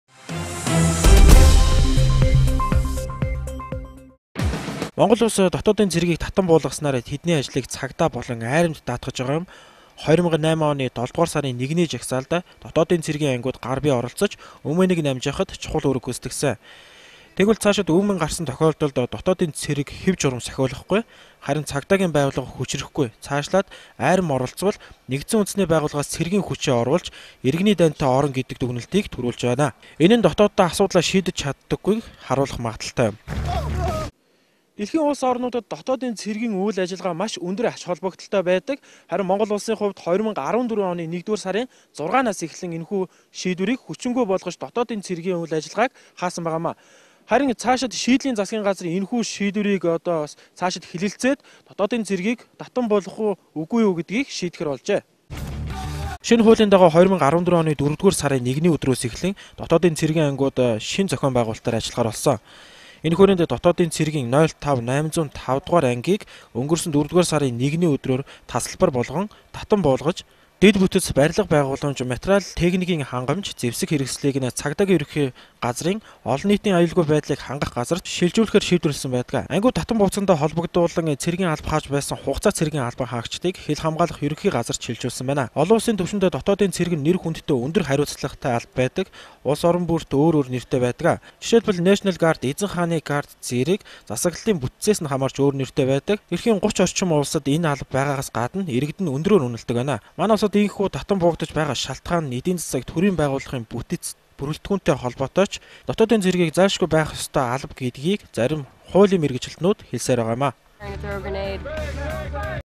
C1 Newsroom - Дотоодын цэргийн тухай хууль
Шинэчлэн баталсан Цагдаагийн албаны тухай хууль хэрэгжиж эхэлснээр өмнө нь мөрдөж байсан Дотоодын цэргийн тухай хууль хүчингүй болсон. ОУ-ын төвшинд дотоодын цэрэг нь үймээн самууныг зогсоож, үндэсний хэв журмыг сахиулдаг үүрэгтэй байдаг.